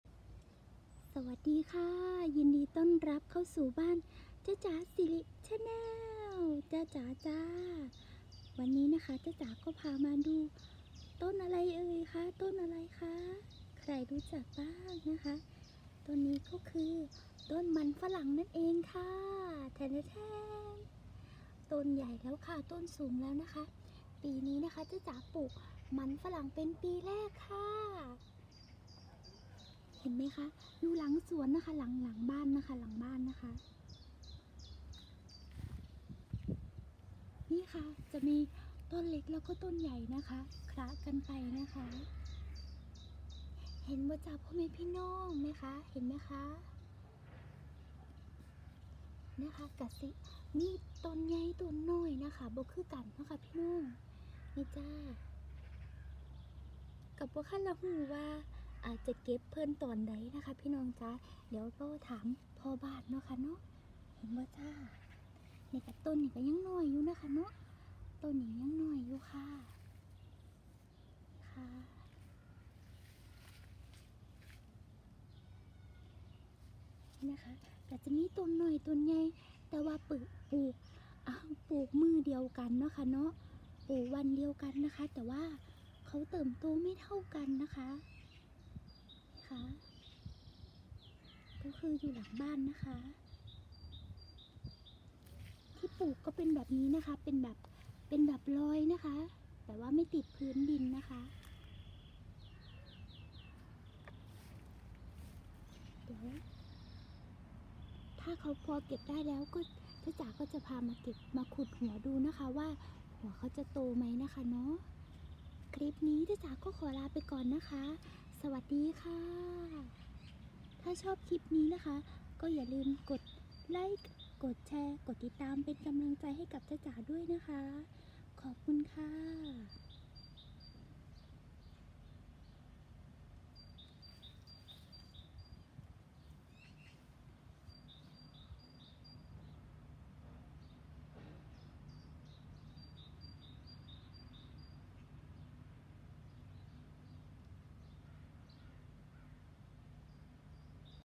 ต้นมันฝรั่งเบลเยี่ยมคะ ต้นมันฝรั่งเพิ่งเคยปลูกเองคะ ท่านใดที่ทราบเวลาเก็บผลผลิตคอมเมนท์ไว้นะคะ
สวัสดีคะ ทุกท่าน วันนี้จ๊ะจ๋าพามาชมต้นฝรั่งที่สวนหลังบ้านคะ ก็จะคล้ายๆกับต้นมันฝรั่งที่อยู่ทางภาคเหนือที่เชียงรายนะคะ แต่ปลูกเองไม่ได้ใส่ปุ๋ยอะไรเลยคะ ต้นก็จะไม่ค่อยโตสม่ำเสมอเท่ากันนะคะ ท่านใดที่ทราบเวลาเก็บหัวมันฝรั่ง คอมเมนท์ไว้ใต้คลิป บอกจ๊ะจ๋าด้วยนะคะ ขอบคุณมากคะ🙏🏼😍ต้นมันฝรั่ง